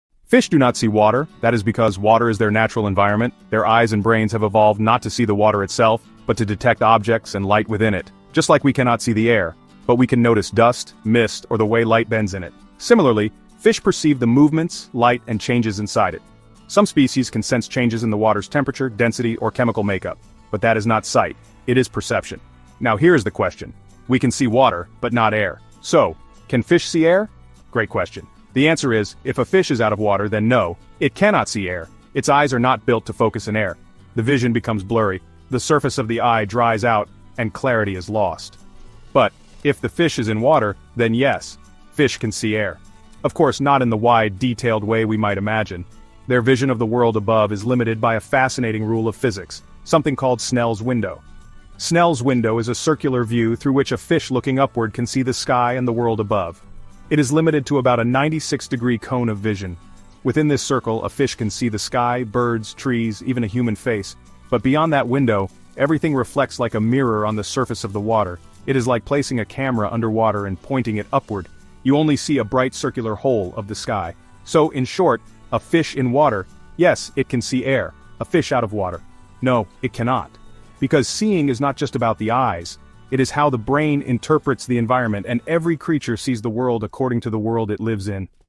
Snell’s Window A Fish’s Secret View of the World
🐠 Can Fish See Air? – Quick Summary
Fish live in water, just like we live in air.
Just as we can’t "see" air, fish don’t really "see" water — they see the objects and light in it.

But can fish see air from underwater?

Yes! Thanks to a phenomenon called Snell’s Window, fish looking upward from underwater can see the sky, clouds, and even things outside the water — but only within a limited circular field of view (about 96° wide).
Outside that window, the surface reflects like a mirror.

However, if a fish is taken out of water, it can’t see air clearly. Its eyes are adapted to water, not air.

In short:
✅ Fish can see air — but only from underwater, and only through a physics-defined window.
❌ Fish cannot see air properly when outside water.